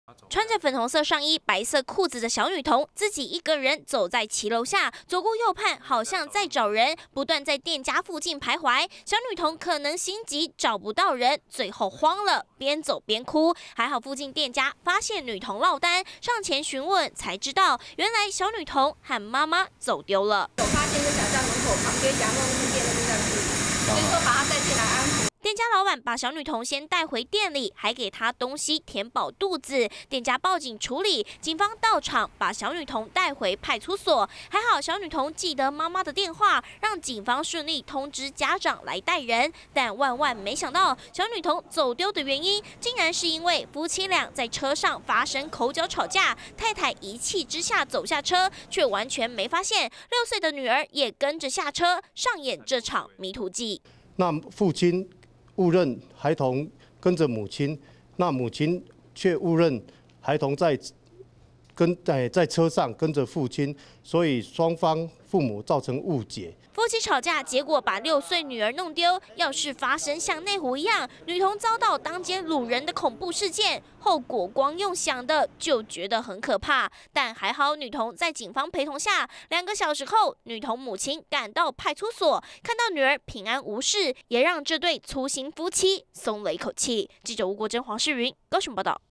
20180814中天新聞 粗心夫妻爆口角 渾然不知搞丟6歲女
真的是一對粗心夫妻!高雄一對夫妻，因為在車上發生口角吵架，結果太太一氣之下走下車，沒有發現6歲的女兒也跟著下車，結果小女童跟不上媽媽腳步，走丟了，在路上邊走邊哭，還好被附近店家報警處理，要是發生擄童事件，落單的女童，恐怕很危險，但所幸最後女童跟著媽媽，平安回家。